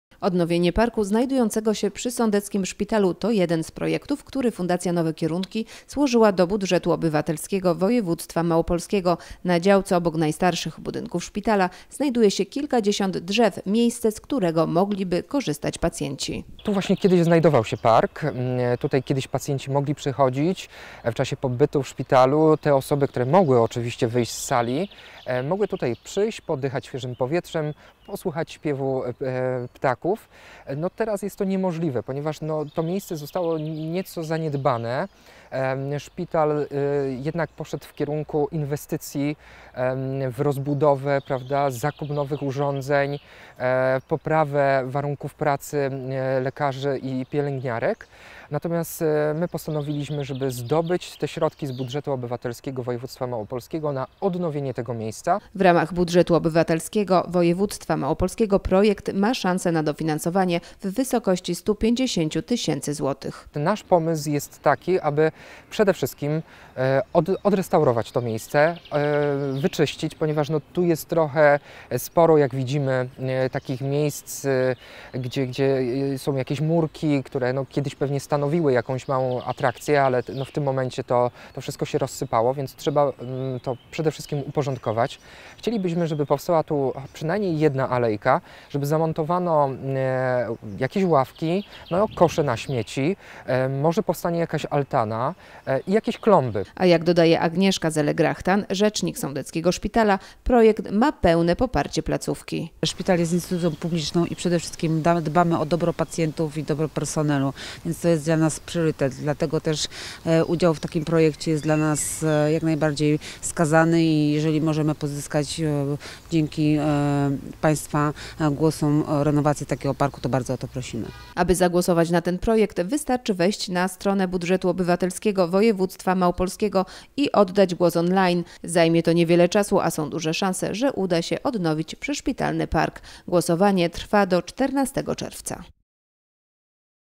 Odnowienie parku przy sądeckim szpitalu, zagłosuj na projekt w ramach BO Województwa Małopolskiego
Odnowienie parku znajdującego się przy sądeckim szpitalu, to jeden z projektów, który Fundacja Nowe Kierunki złożyła do Budżetu Obywatelskiego Województwa Małopolskiego. Projekt zakłada rewitalizację parku i stworzenie miejsca przyjaznego pacjentom. Na działce, tuż obok najstarszych budynków placówki znajduje się kilkadziesiąt drzew zachęcających do spacerów, obcowania z naturą czy przebywania na świeżym powietrzu. Brakuje jednak ławek, ścieżki, koszy na śmieci oraz nowych krzewów i kwiatów. Zapewniając to wszystko, park może stać się miejscem odpoczynku i oderwania się od szpitalnej codzienności dla pacjentów, osób odwiedzających chorych, a także dla czekających na badania lub wyniki. Głosowanie w ramach tegorocznej edycji Budżetu Obywatelskiego Województwa Małopolskiego już ruszyło. Szczegółowy opis projektu „Kierunek: Nowy Sącz z nowym parkiem – rewitalizacja terenu przy sądeckim szpitalu” znajduje się na stronie internetowej fundacji
Głosować można za pośrednictwem strony Urzędu Marszałkowskiego w Krakowie malopolska.budzet-obywatelski.eu/glosowanie
Głosowanie potrwa do 14 czerwca.